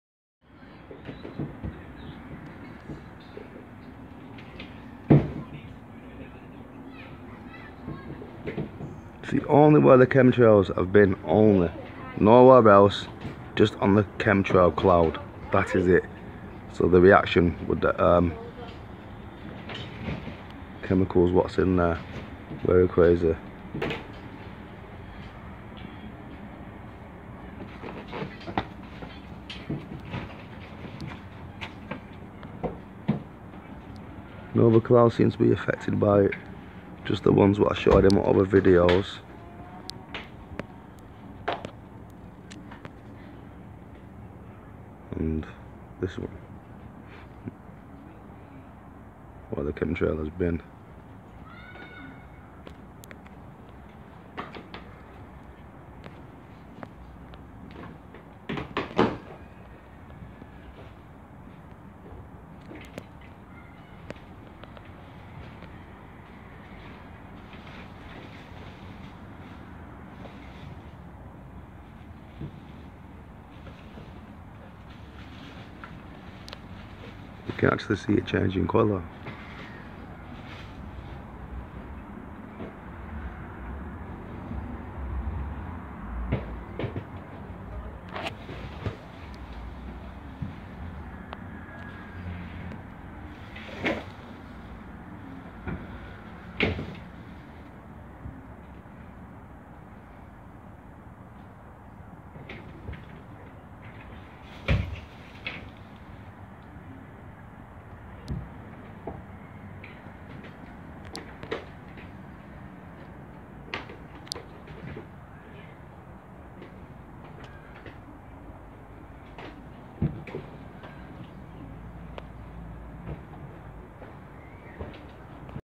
Chemtrail only affected
The chemtrail seem to be the only clouds affected with this strange light or rainbow what ever they are doing is causing a reaction and creating some sort of strange light do you agree share subscribe and leave a comment guys